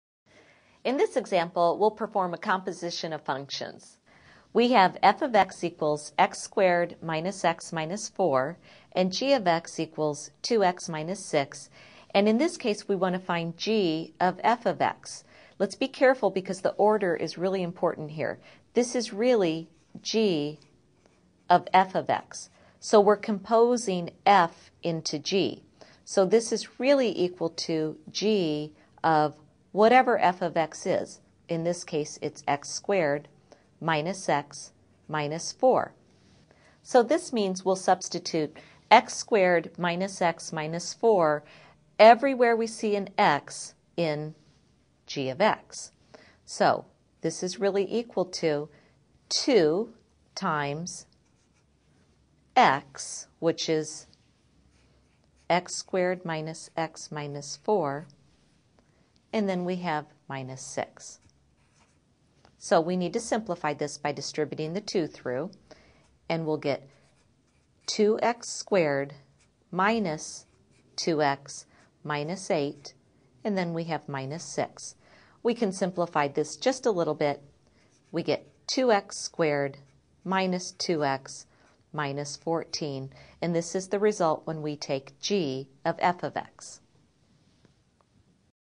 Chapter Test Prep – Ch 2, Exercise 21
An instructor walks you step-by-step through the solution to Exercise Y in the Chapter X Test from the textbook Blitzer College Algebra 8e.